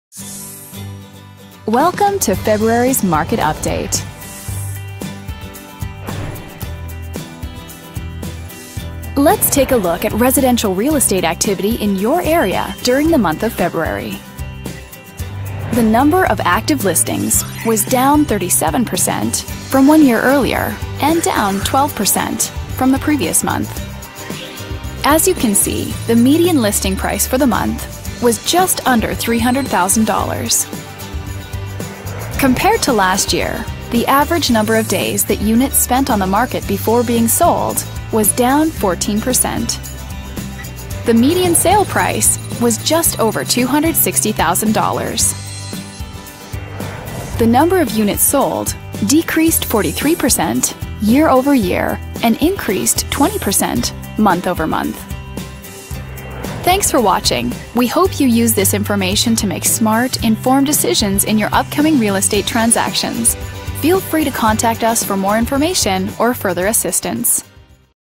92102 - San Diego,CA, Real Estate Market Update from Willis Allen,February,2014
Willis Allen Video Update recapping market activity within 92102 - San Diego, CA, during the month of February, 2014